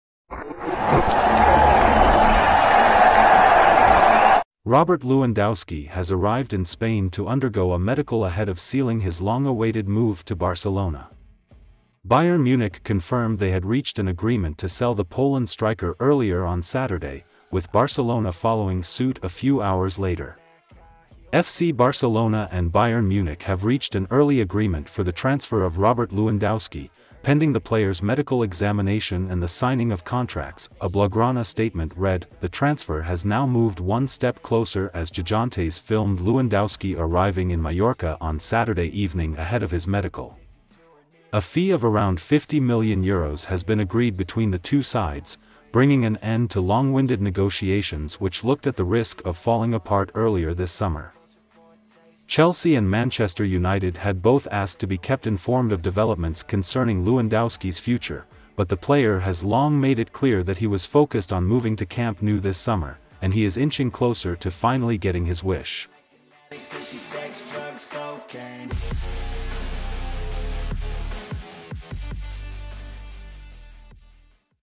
Lewandowski Arrives In Spain For Barcelona Medical
Robert Lewandowski has arrived in Spain to undergo a medical ahead of sealing his long-awaited move to Barcelona.

Bayern Munich confirmed they had reached an agreement to sell the Poland striker earlier on Saturday, with Barcelona following suit a few hours later.